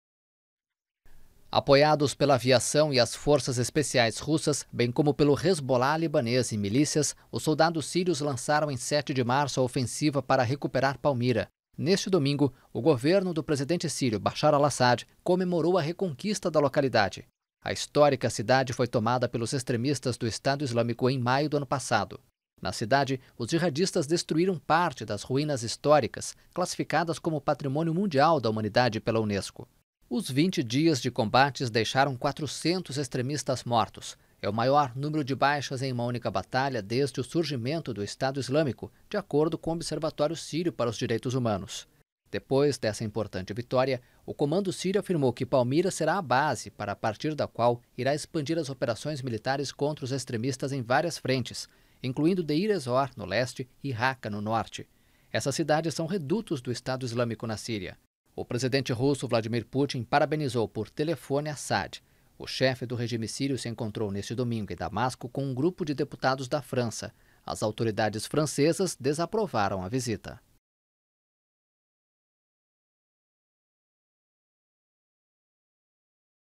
Síria retoma Palmira e impõe grande derrota ao EI
O exército sírio, apoiado pela Rússia, impôs uma importante derrota neste domingo ao grupo Estado Islâmico. O regime de Bashar al-Assad recuperou o controle da cidade de Palmira, e prometeu expulsar a organização extremista de seus principais redutos na Síria.